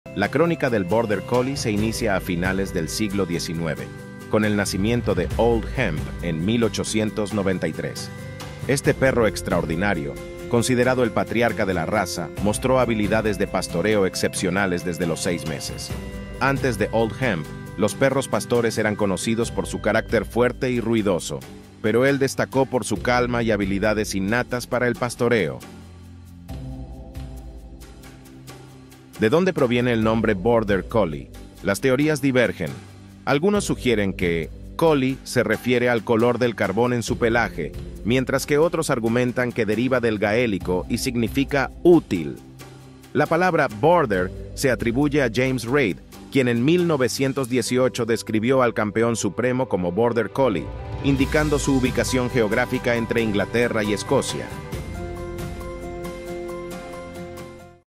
El Perro Más Inteligente del Mundo
🐾 En este episodio de Cultura Peluda, nos adentramos en el asombroso universo del Border Collie, considerado el perro más inteligente del mundo. Desde su intrigante origen hasta sus características físicas y cualidades únicas, descubre por qué estos compañeros peludos son tan especiales. 🐶💡 Prepárate para un viaje lleno de curiosidades caninas y conoce todo sobre la raza líder en inteligencia según Stanley Coren.

by. D'monte ®